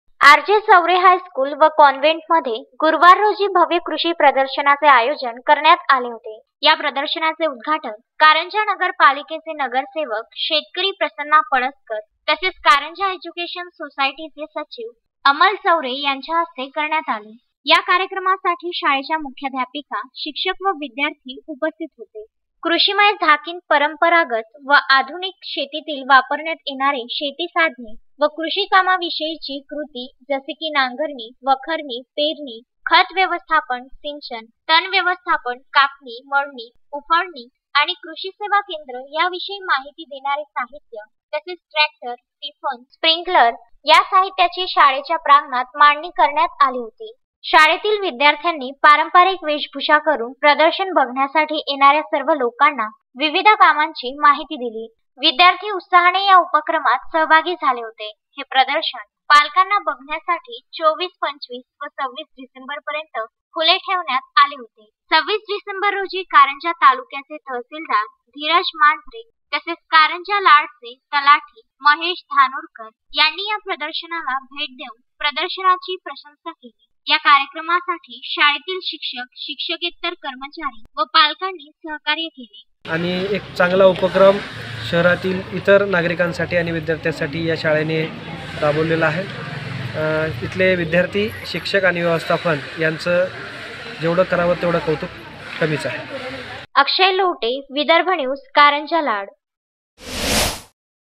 KARANJA LAD | चवरे हायस्कूल मध्ये कृषी प्रदर्शिनीचे आयोजन
विदर्भन्यूज365मराठी चवरे हायस्कूल मध्ये कृषी प्रदर्शिनीचे आयोजन

शेतकरी दिना निमित्त भरले कृषी प्रदर्शन